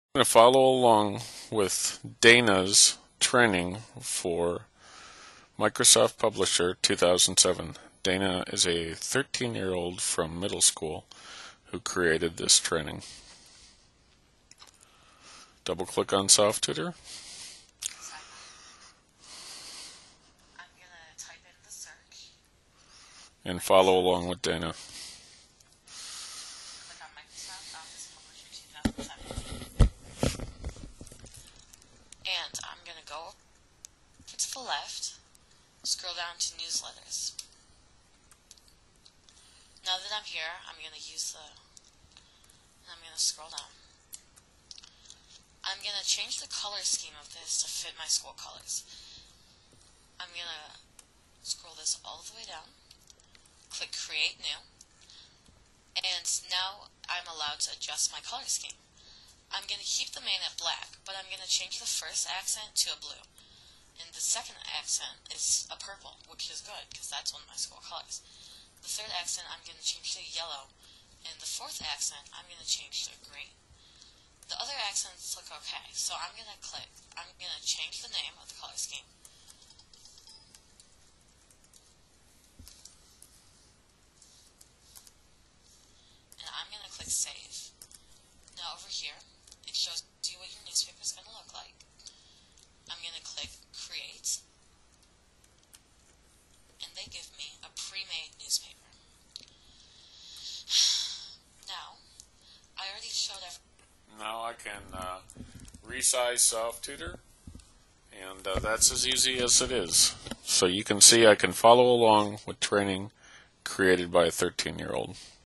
We try Dana's SofTutor Training
We follow along with some training made by an 8th grader to help her friend with the school paper